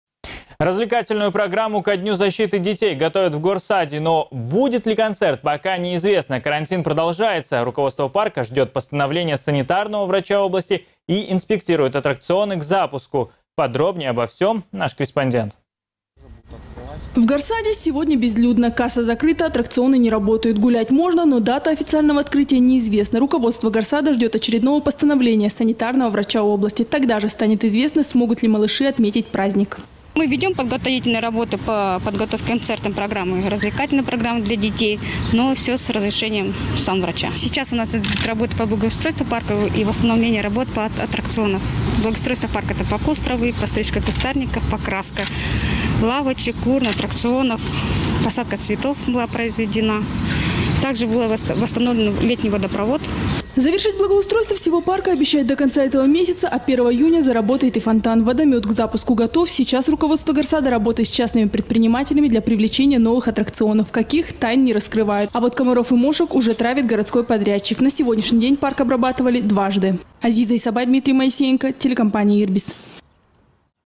Развлекательную программу ко Дню защиты детей готовят в горсаде 25.05.20
Но будет ли концерт, пока не известно. Карантин продолжается — руководство парка ждет постановления санитарного врача области и инспектирует аттракционы к запуску.